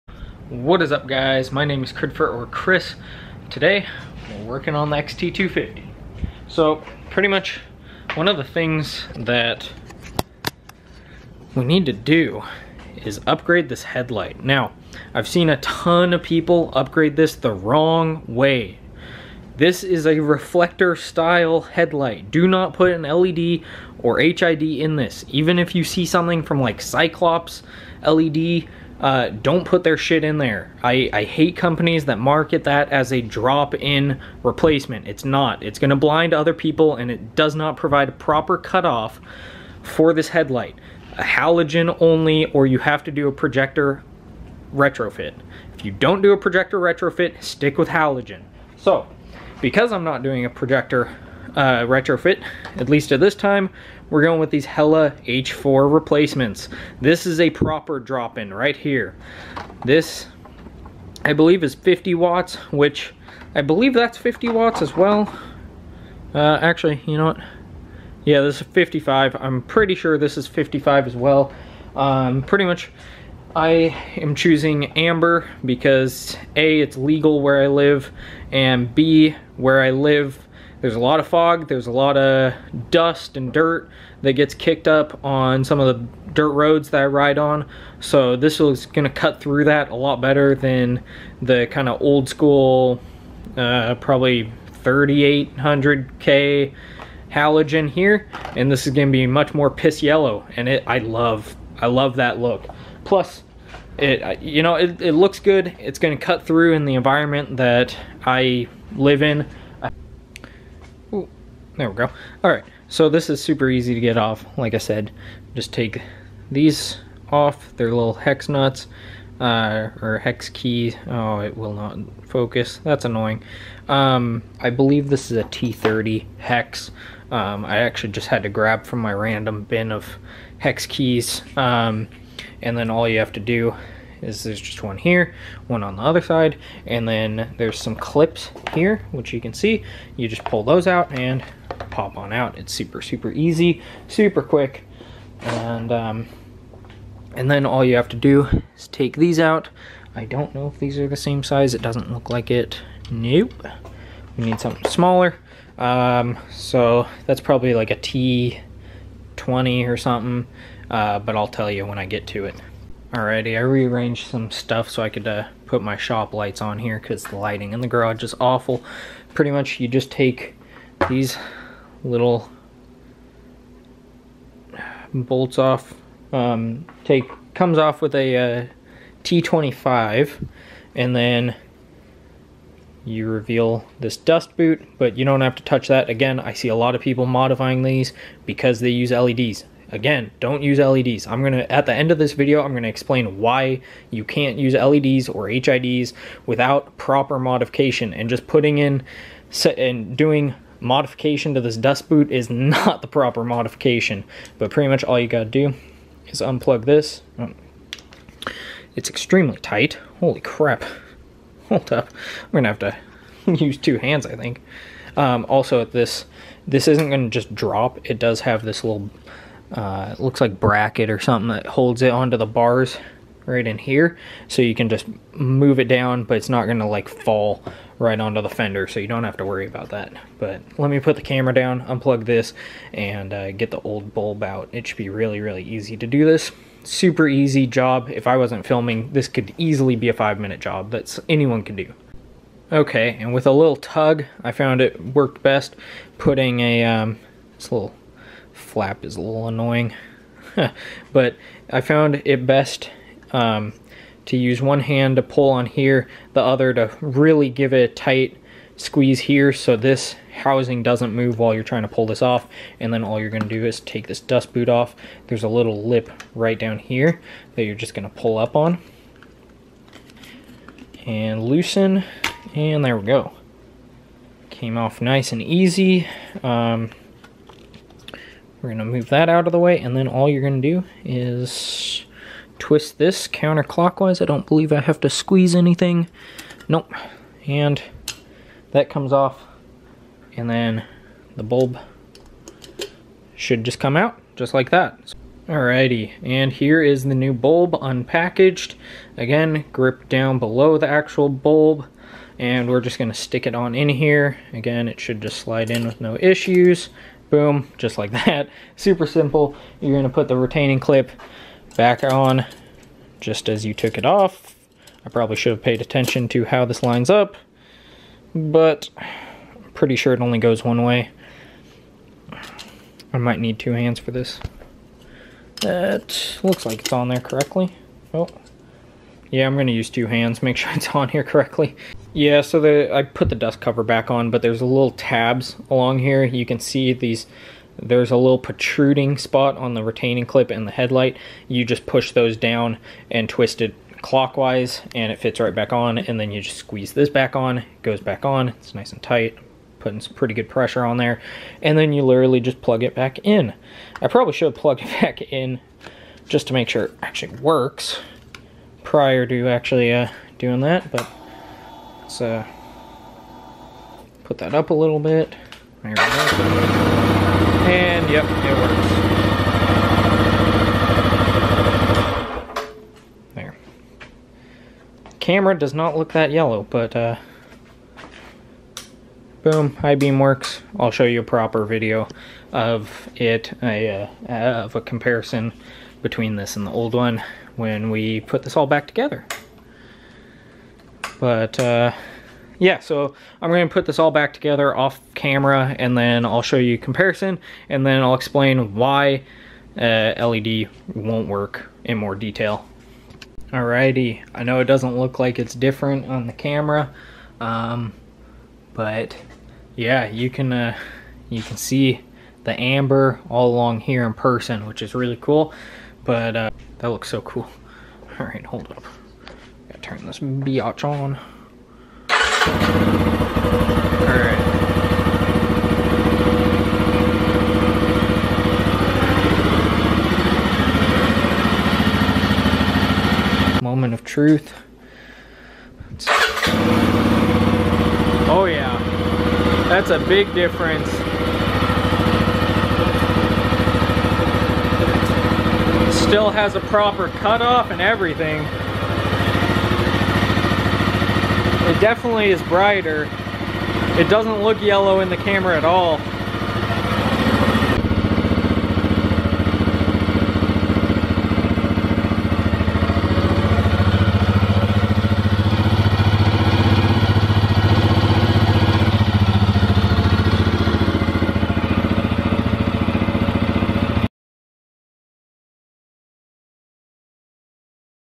2020 Yamaha XT250 Headlight Upgrade
Today we work on the XT250 and put a Hella amber replacement bulb in.

Hella H4 Amber Bulb

Wheel Well: Christopher Ostrem

Music
Intro: [Song] By [Artist]
Backing Track: [Song] By [Artist]